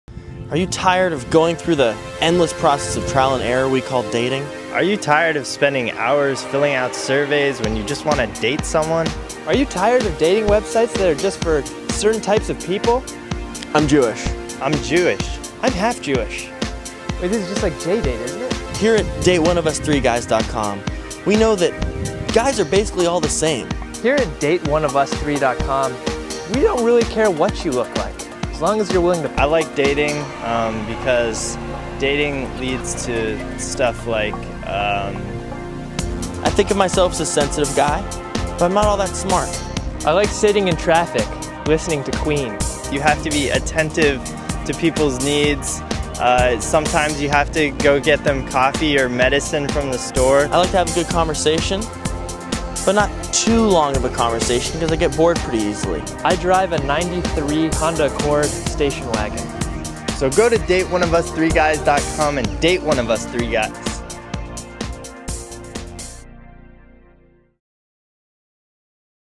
Date One Of Us Three Guys
A revolutionary new dating website makes the process simple and effective.